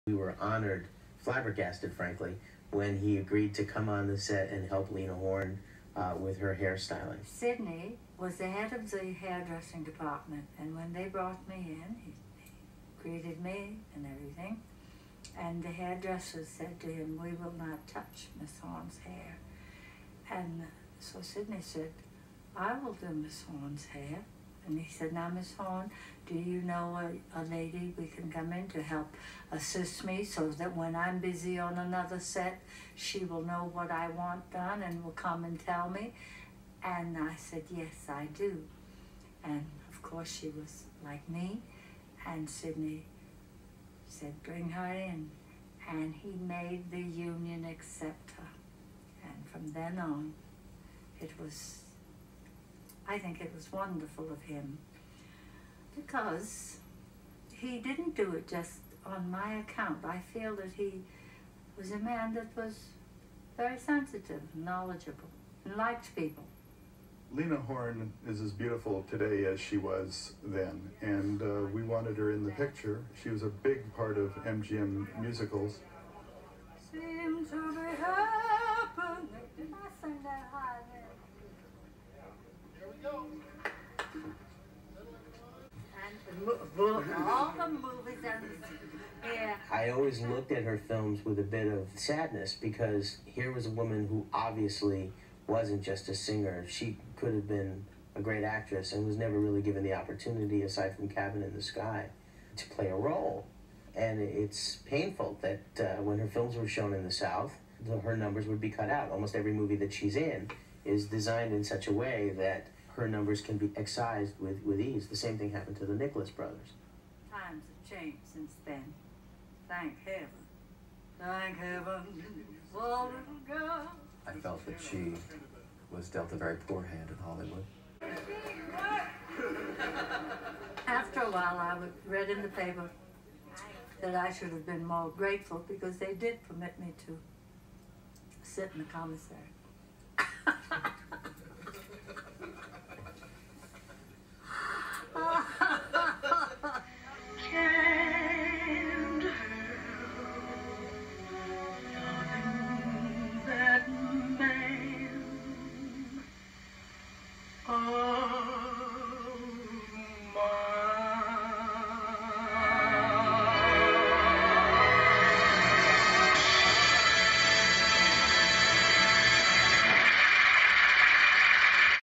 Lena Horne on the racism at MGM
Lena Horne, a true musical talent, and the racism she experienced behind the scenes at MGM, where she was the first Black person to have sighed a film studio contract.